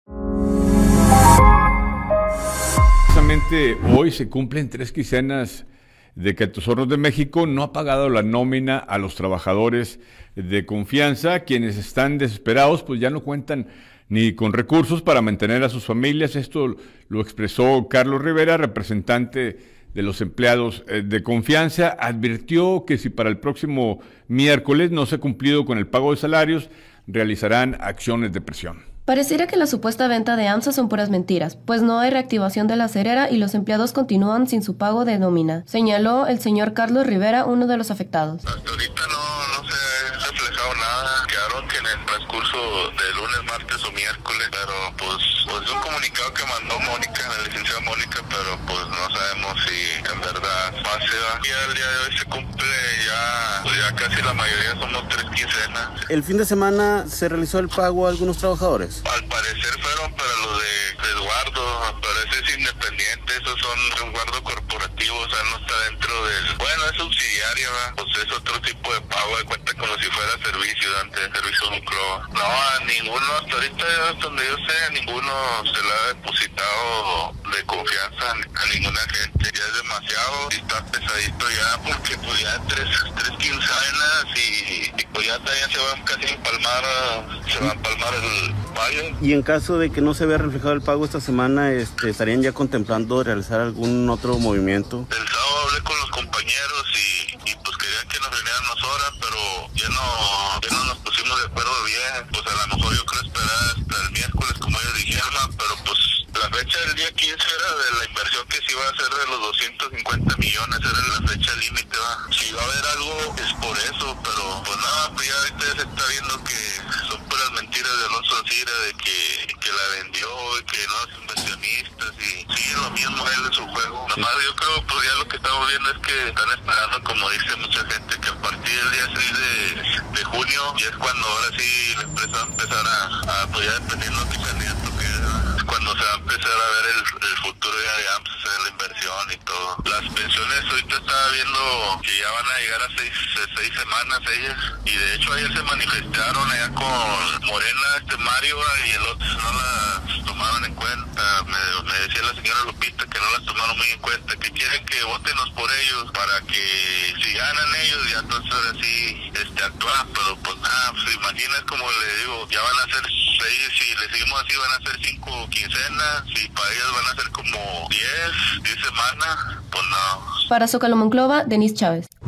Se cumplen tres quincenas de que AHMSA no ha pagado la nómina a los empleados de confianza
Se cumplen tres quincenas de que Altos Hornos de México no ha pagado la nómina a los empleados de confianza, quienes están desesperados pues ya no cuentan con recursos para mantener a sus familias, expresó Carlos Rivera, representante de los trabajadores.
Comentó que, si para el miércoles no se ha cumplido con dicho pago, realizaran acciones de presión.